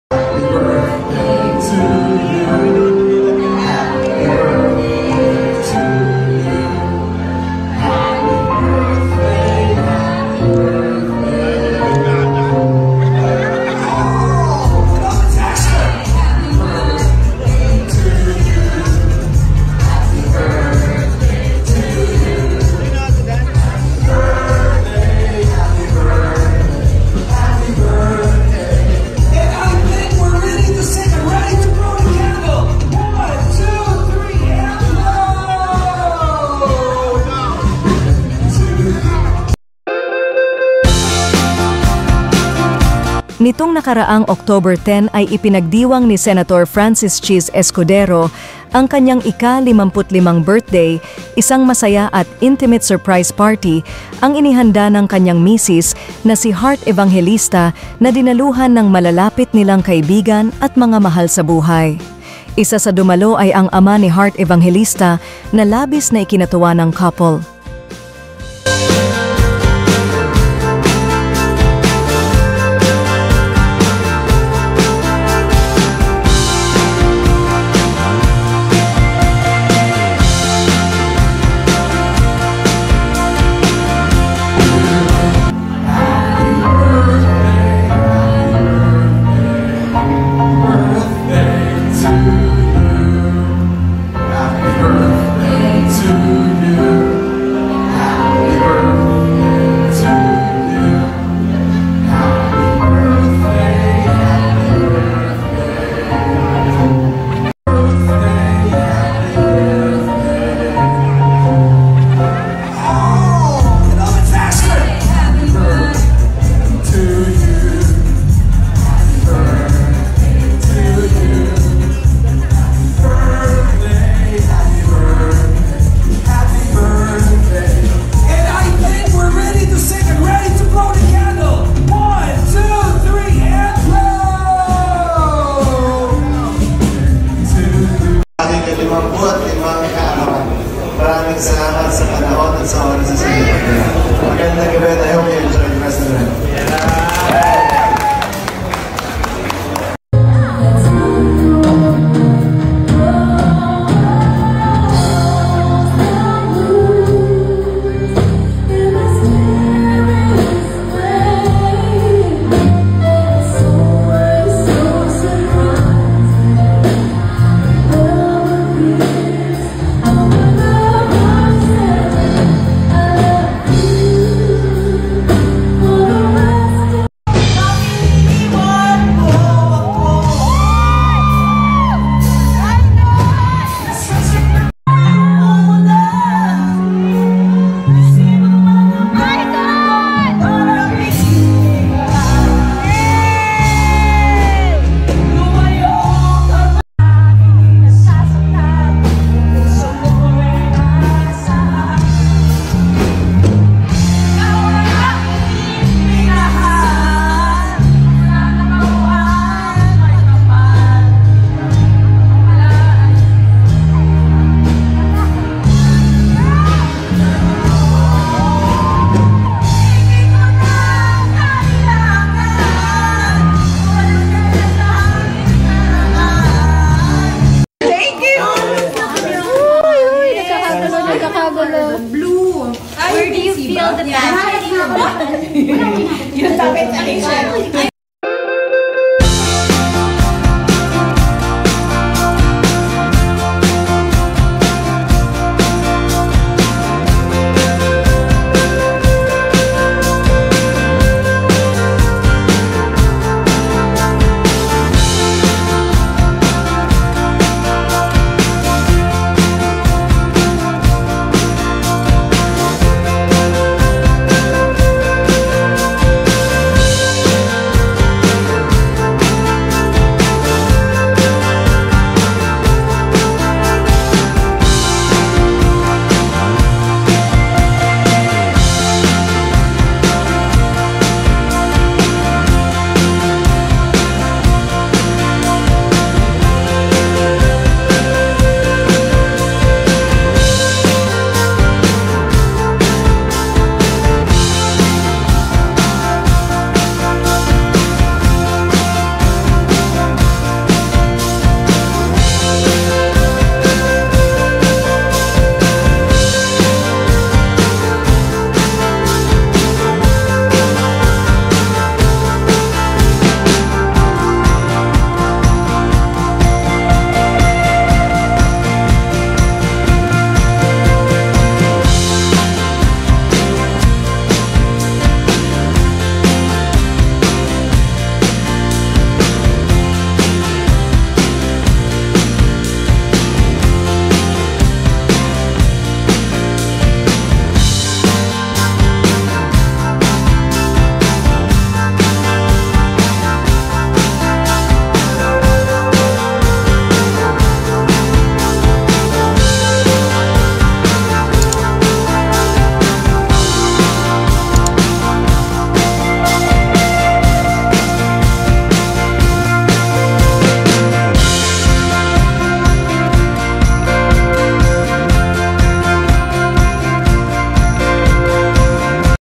Sen Chiz Escudero 55th Birthday Di Kinaya SORPRESA ni Heart Evangelista
Sen Chiz Escudero 55th Birthday